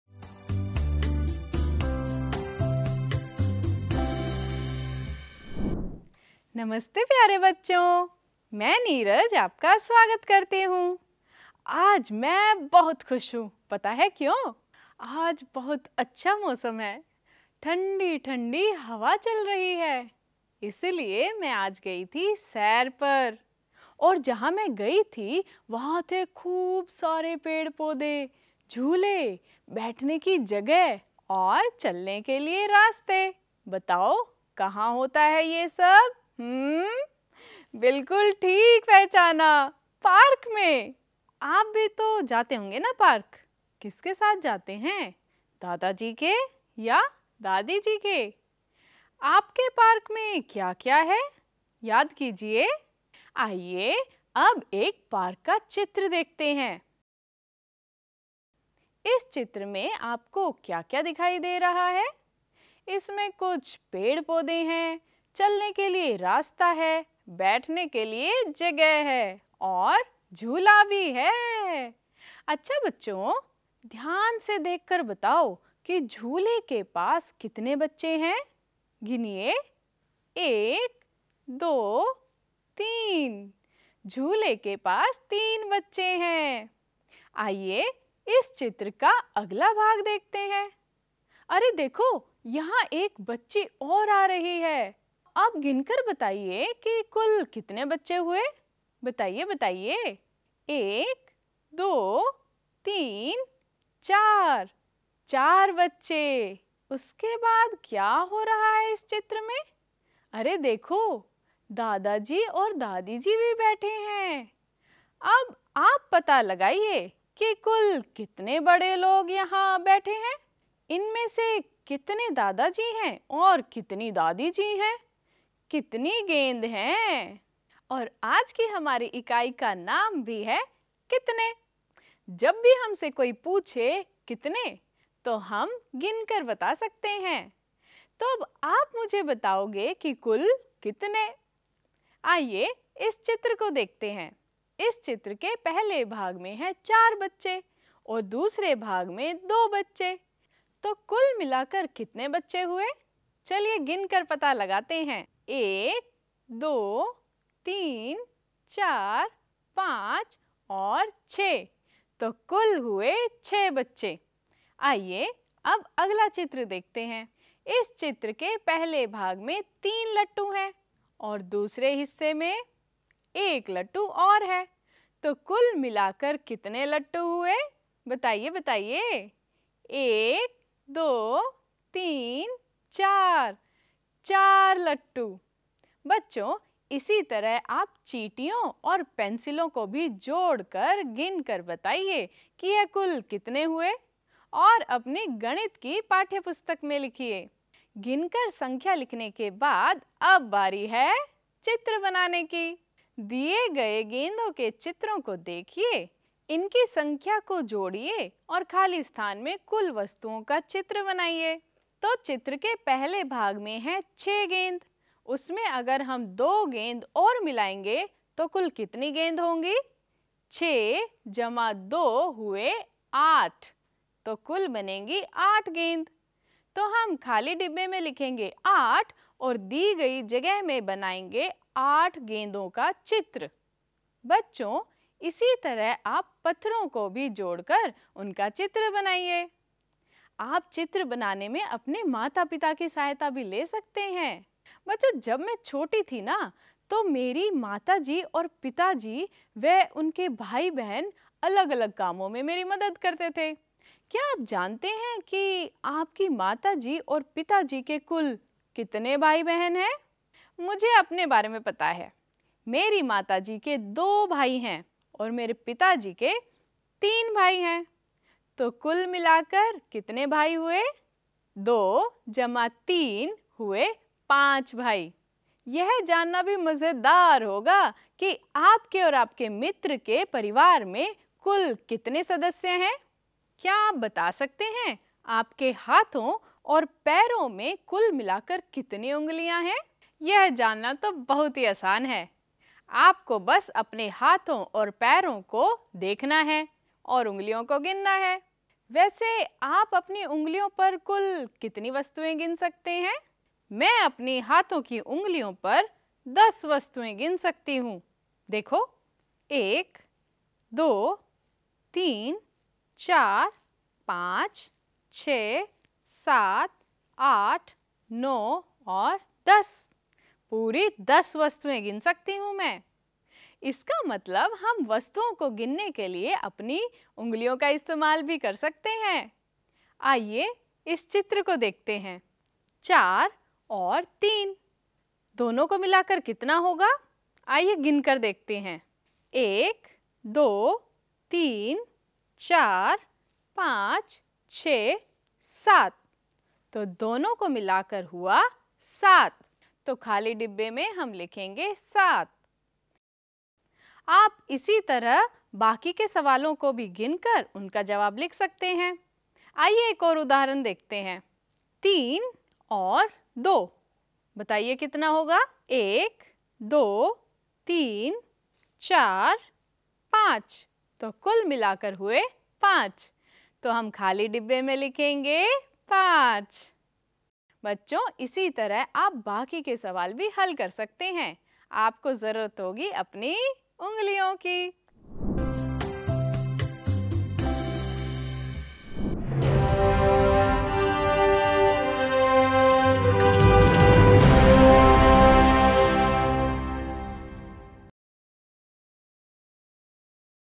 NCERT कक्षा-1 गणित पाठ- कितने- भाग-1
यह एनसीईआरटी पाठ्यपुस्तक पर आधारित कक्षा 01 के लिए हिंदी माध्यम में गणित का एक वीडियो कार्यक्रम है